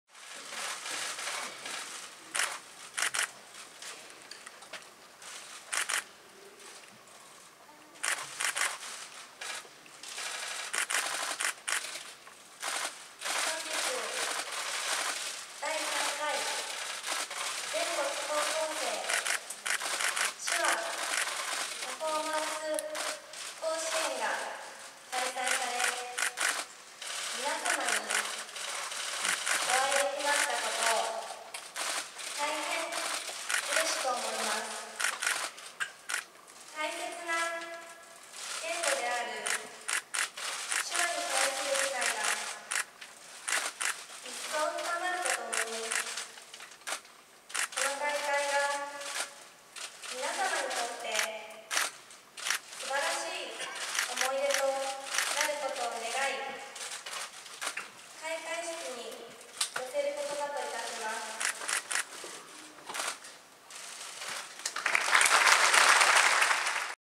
佳子さま、手話であいさつ
鳥取県を訪問中の秋篠宮ご夫妻の次女佳子さまは２５日、倉吉市内で開催された「第３回全国高校生手話パフォーマンス甲子園」に出席された。
    開会式で、佳子さまは手話を交えて「大会を通して聴覚に障害がある方々と、大切な言語である手話に対する理解が一層深まることを願います」などとあいさつした。続いて、手話を用いた聴覚障害者との交流を描いた劇や歌、ダンスなど高校生によるパフォーマンスを観賞した。
【時事通信大阪支社撮影】